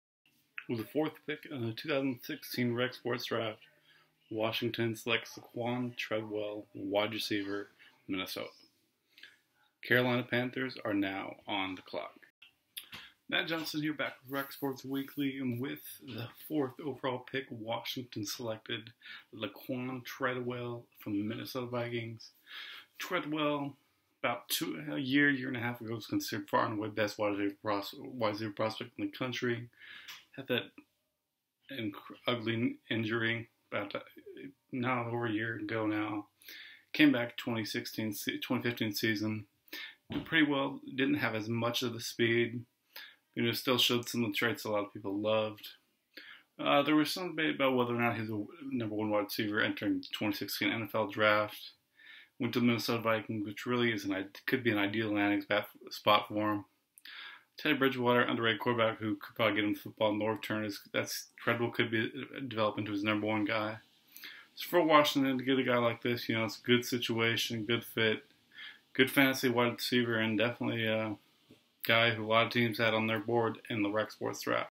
REC Sports Draft: 1.04 Washington selects Laquon Treadwell, Analysis
With the fourth pick in the 2016 REC Sports Draft, Washington select Laquon Treadwell, WR, Minnesota